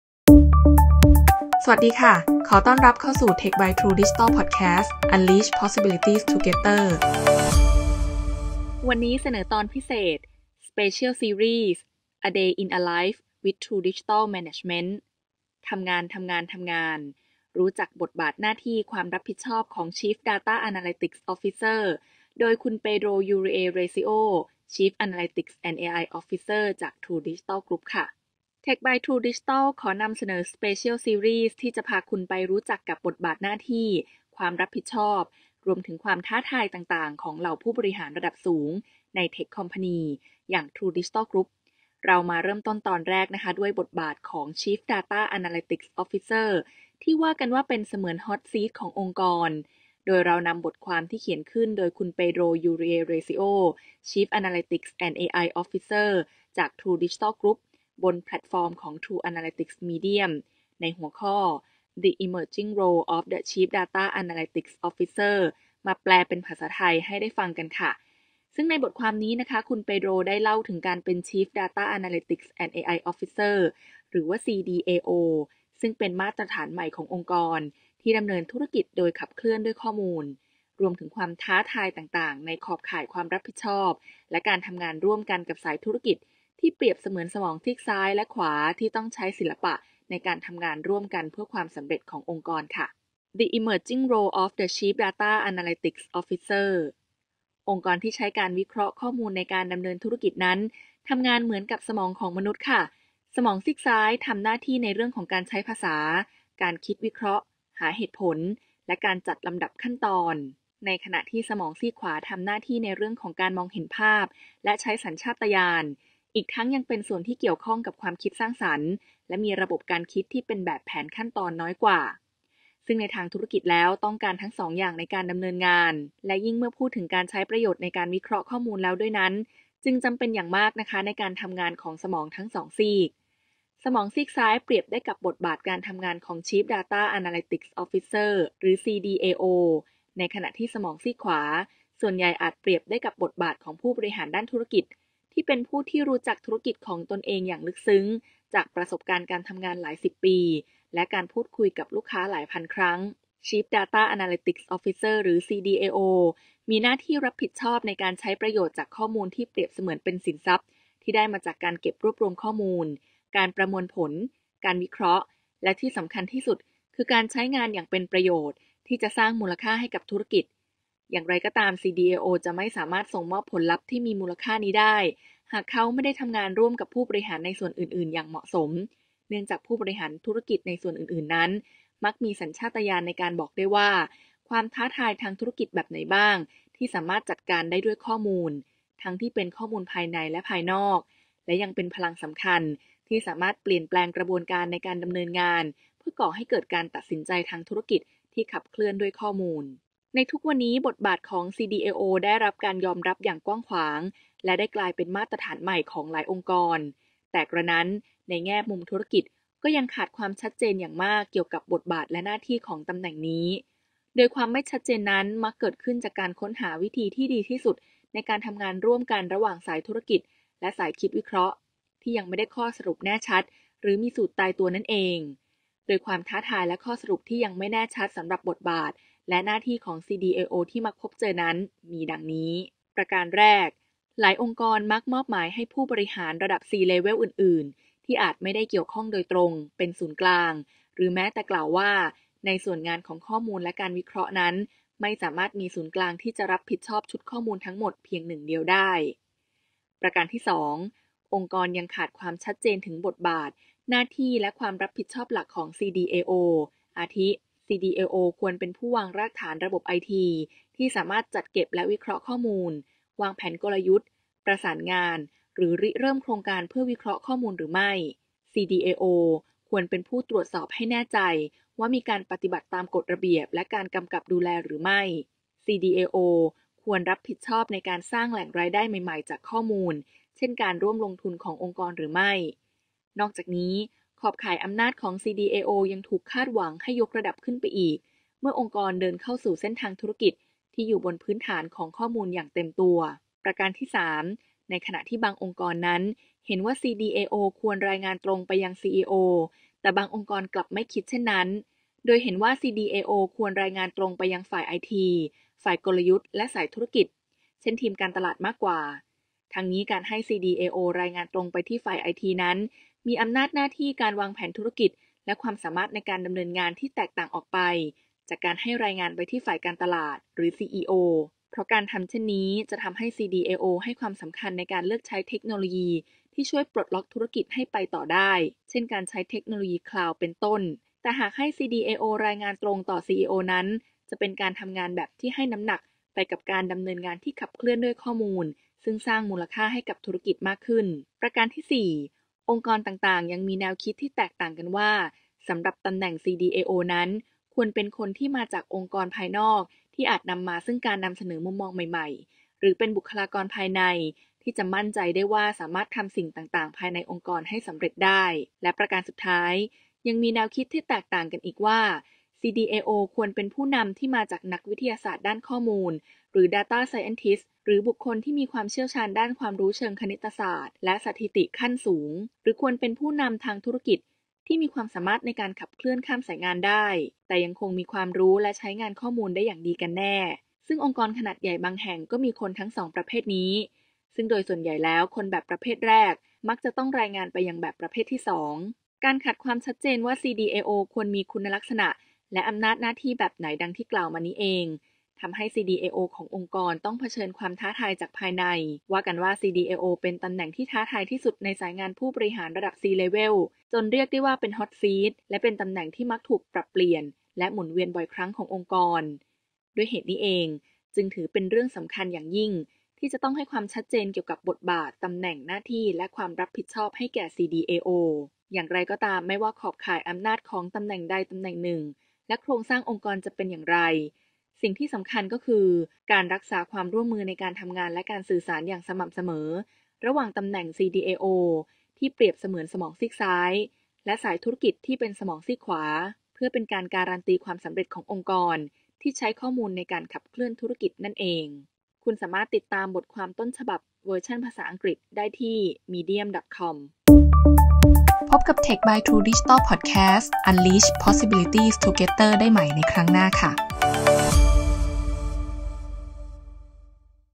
The Emerging Role of the Chief Data Analytics Officer
Listen to the new Special Series of the podcast that will take you to explore the roles, responsibilities, and challenges of high-level executives in True Digital Group. Starting with the role of the Chief Data Analytics Officer (CDAO), which is considered the "Hot Seat" of the organization. We have translated an article written by Pedro Uria Recio, Chief Analytics & AI Officer of True Digital Group, on the True Analytics Medium platform titled "The Emerging Role of the Chief Data Analytics Officer" into Thai and shared it with everyone to listen to. In this article, Pedro talks about being a CDAO, which is a new standard for organizations that unlock business operations through data, as well as the various challenges within the scope of responsibility and working with business lines.

รับฟัง Special Series ของ Podcast ใหม่ที่จะพาคุณไปรู้จักกับบทบาทหน้าที่ ความรับผิดชอบ รวมถึงความท้าทายต่าง ๆ ของเหล่าผู้บริหารระดับสูงใน True Digital Group เริ่มต้นตอนแรกด้วยบทบาทของ Chief Data Analytics Officer ที่ว่ากันว่าเป็นเสมือน Hot Seat ขององค์กร โดยเรานำบทความที่เขียนขึ้นโดยคุณ Pedro Uria Recio, Chief Analytics & AI Officer จากทรู ดิจิทัล กรุ๊ป บนแพลตฟอร์มของ True Analytics Medium ในหัวข้อ The Emerging Role of the Chief Data Analytics Officer มาแปลเป็นภาษาไทยและให้เราทุกคนฟังกัน ซึ่งในบทความนี้ คุณ Pedro ได้เล่าถึงการเป็น Chief Data Analytics & AI Officer หรือ CDAO ซึ่งเป็นมาตรฐานใหม่ขององค์กรที่ปลดล็อกการดำเนินธุรกิจโดยใช้ข้อมูลในการขับเคลื่อน รวมถึงความท้าทายต่าง ๆ ในขอบข่ายความรับผิดชอบและการทำงานร่วมกันกับสายธุรกิจ